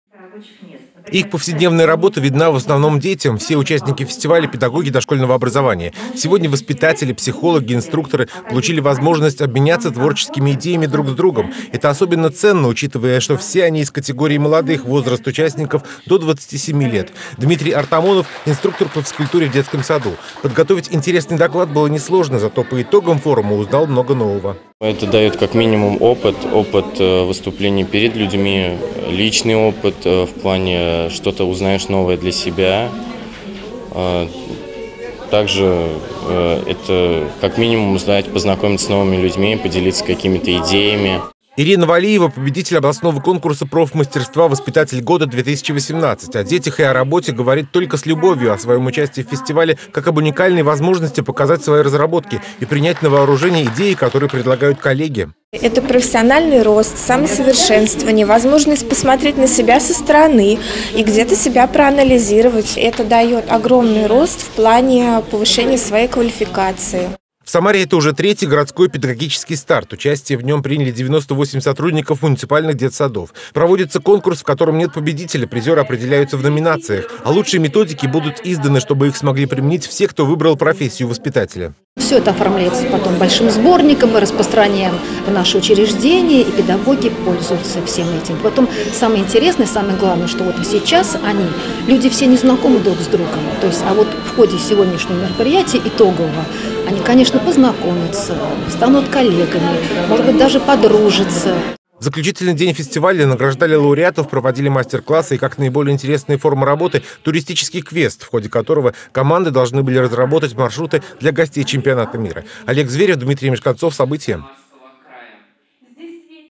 В Самаре подвели итоги фестиваля "Педагогический старт"
Туристический квест для гостей чемпионата мира. Участники городского фестиваля "Педагогический старт" выполнили финальное задание. Форум молодых педагогов собрал 98 участников - сотрудников детских садов города. Олег Зверев присутствовал на подведении итогов.